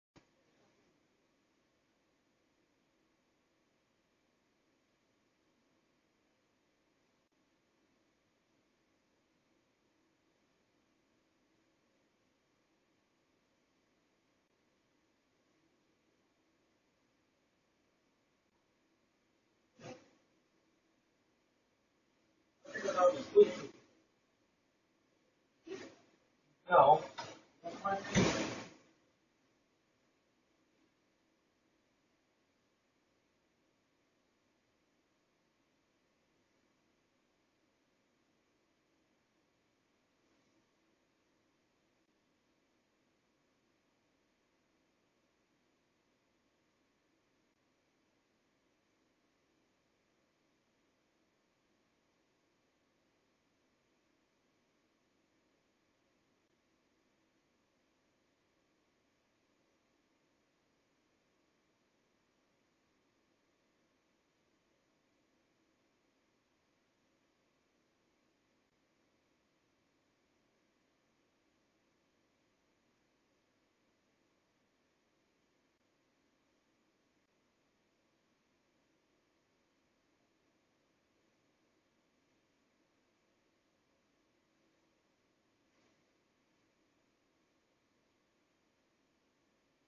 Look how it's raining outside
Capture by the Ring stick up cam. looks like some heavy rain fallen.